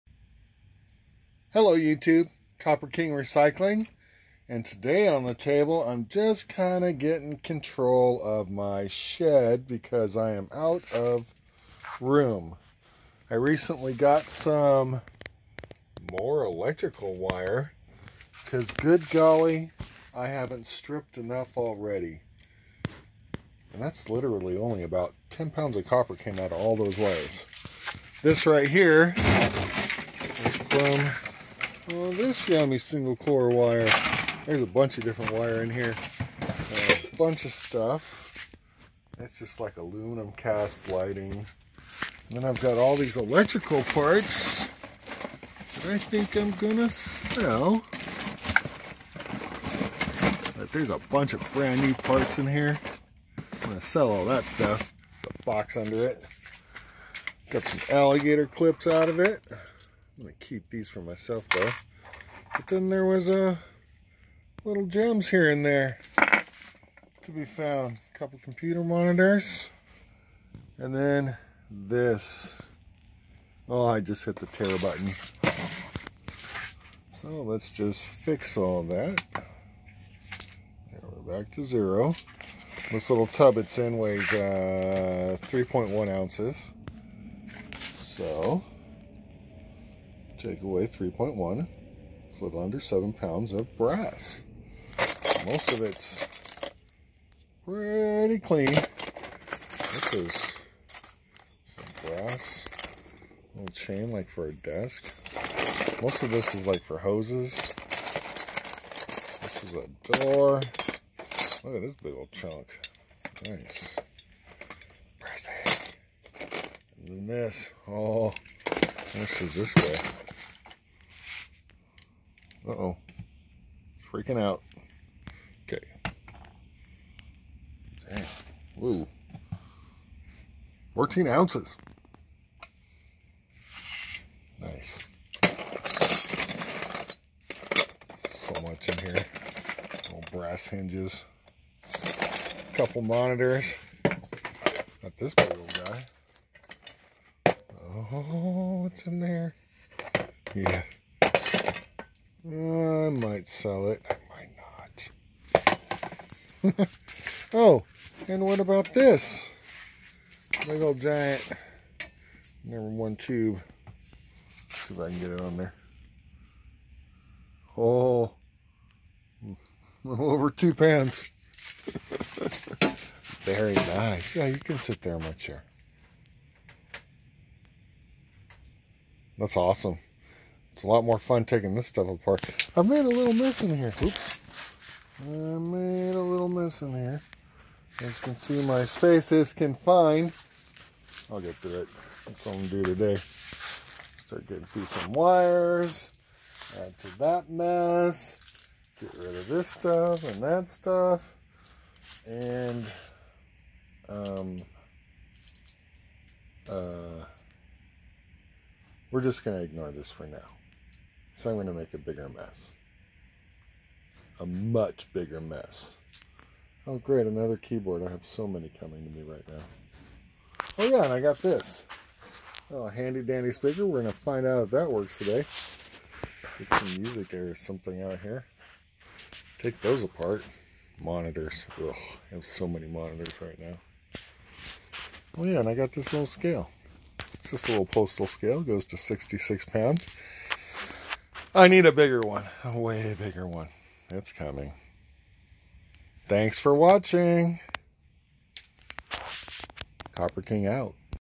Free Brass. Volunteer Work That Pays In Scrap Metal.
I was helping out Pastor Carolyn M. with some free labor. All I had to do was help her Husband Mike move some music equipment from one place to another.  No problem. During the work, Pastor Carolyn said, you can have whatever is in the garage. Anything left behind is garbage. I found a bunch of brass, some copper.  A whole mess of wires, some copper gaiter clips and more old computer monitors.

I've got to make space, because I'm getting more E-waste for helping out. Awesome for me but I'm definitely creating another Scrapalanche.